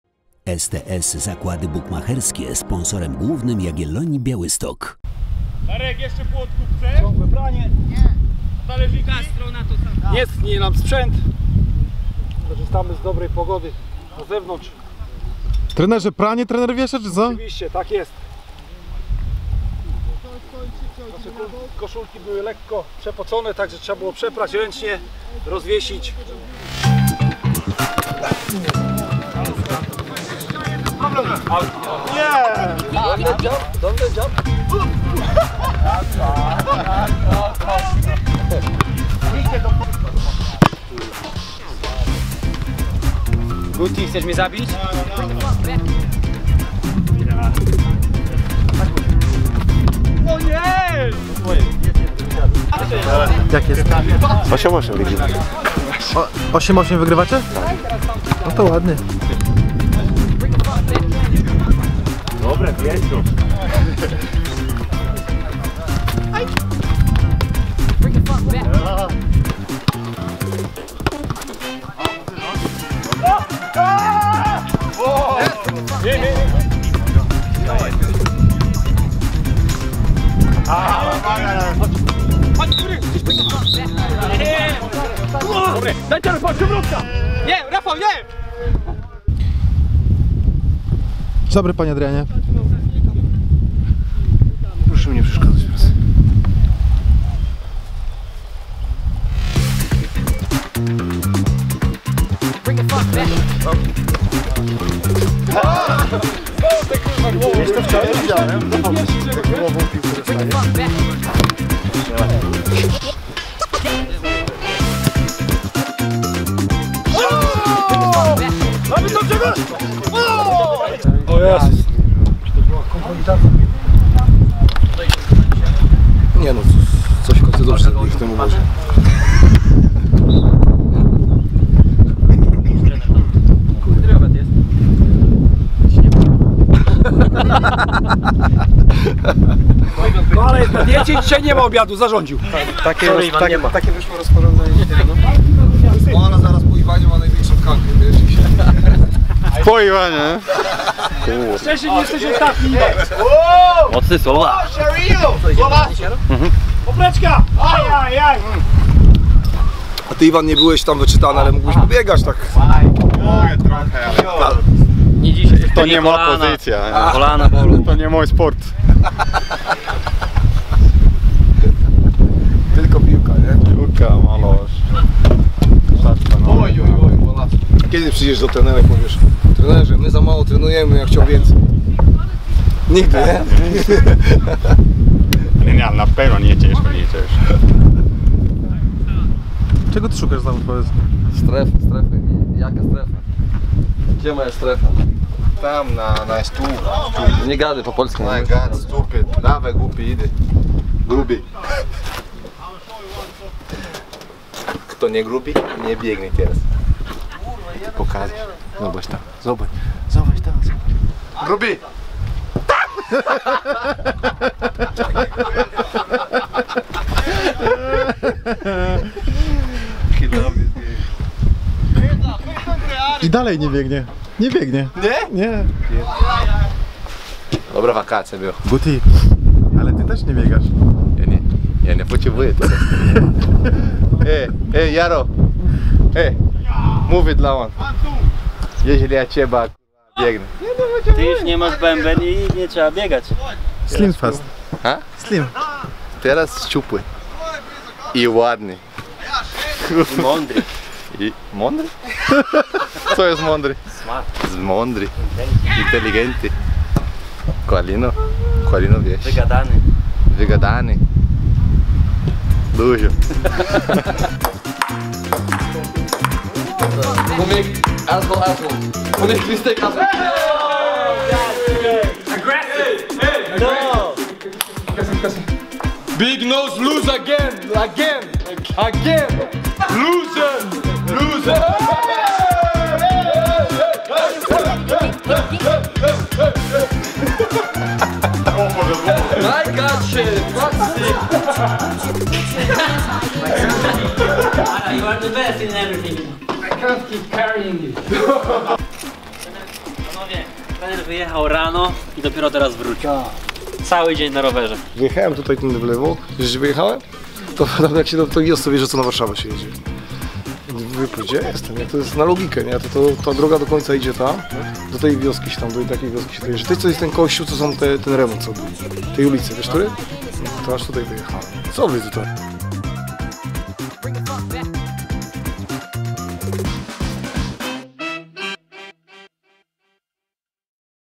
Czwartek na sportowo
Czwartek był dniem pod znakiem sportu. W wolnych chwilach nasi piłkarze grają w tenisa stołowego, jeżdżą na rowerach... a niektórzy biegają :)